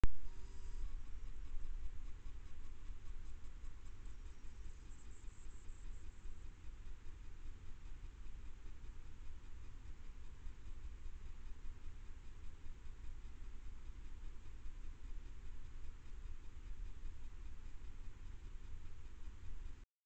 Showdown at the Hedgehog Corral Episode 1
Our friendly hedgehog comes by early in the evening for his supper, and finds it not so easy to get into the feeding station. What's stopping him from going in?